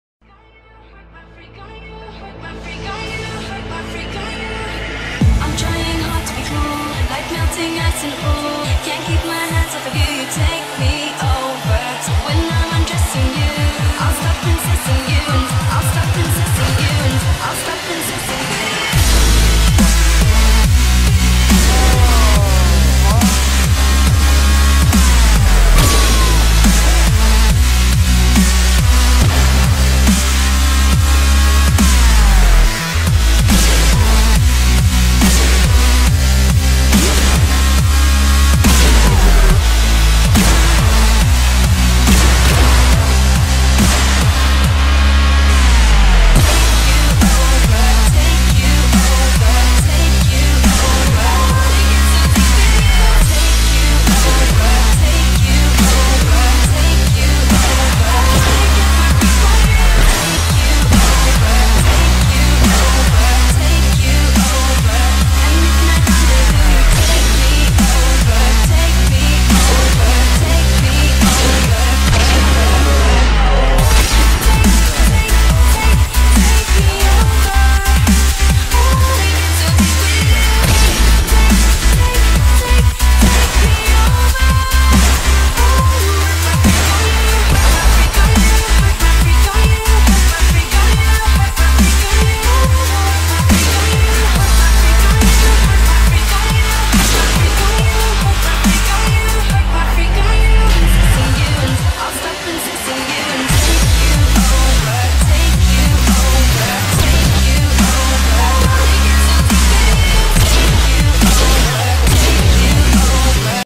Text DnB and R7 Tru Dual Episode
Hey everyone here is a sick dual episode from DnB and Tru. This episode is really sick with a pretty even mixture of trickshots and feeds. A few of the clips are fairly old so the trickshot standards werent too high. However this episode was still insane. eRa Gamble did an amazing job on the edit as well so go check out his channel. I hope you all enjoy leave some feedback in the comments below. Lets break 90 likes for this sick dual episode!